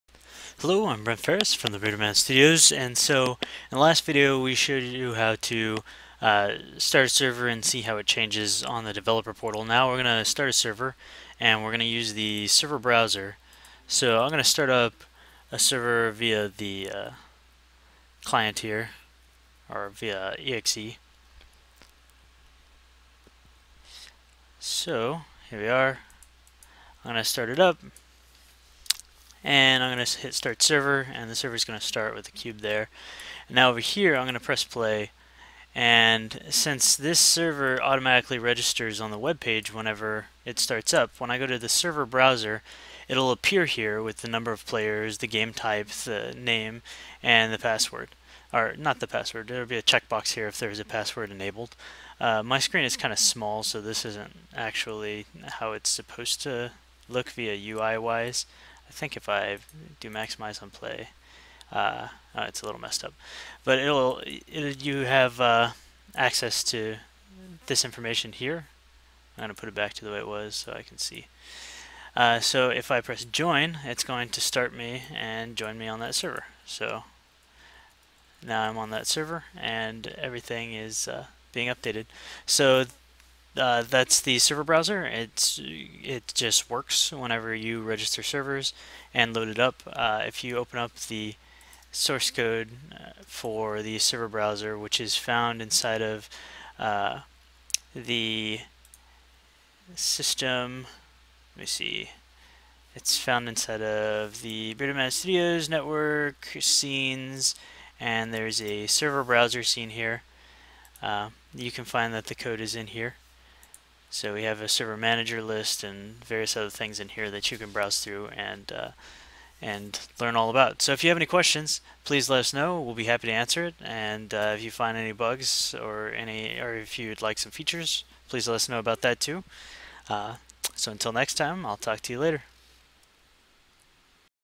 Forge Networking Server Browser
This video is an overview of the server browser functionality for the Arbiter master server.